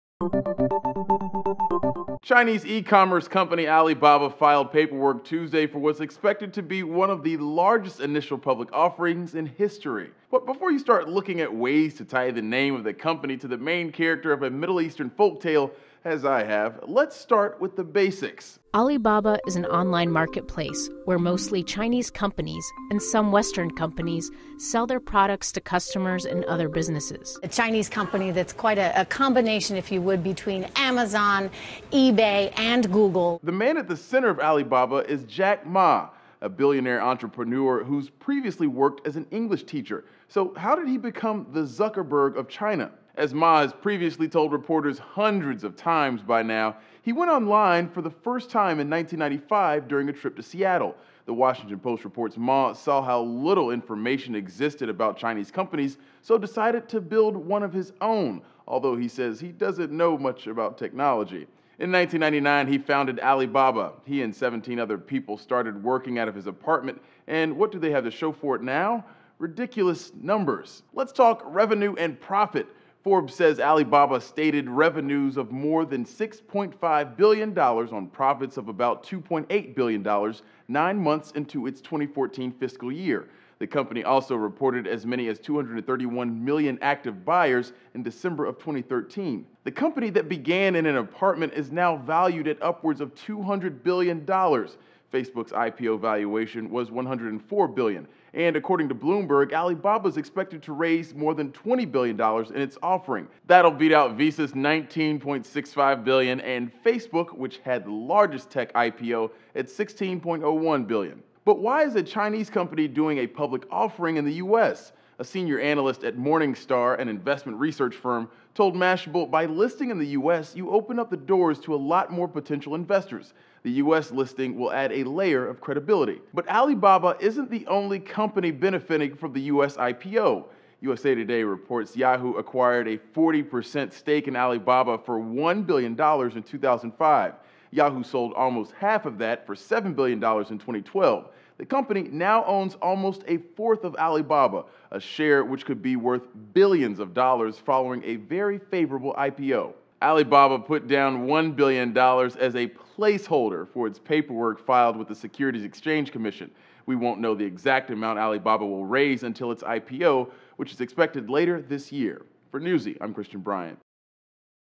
Chinese Company Alibaba Could Have A Record-Setting U.S. IPO
The Chinese e-commerce company may not have widespread name recognition in the U.S., but it's U.S. IPO could be the largest ever.

Sources:
Wikipedia
The New York Times
Bloomberg
ABC
The Washington Post
Forbes
Bloomberg
Mashable
Flickr
USA Today

Image via: Bloomberg / Tomohiro Ohsumi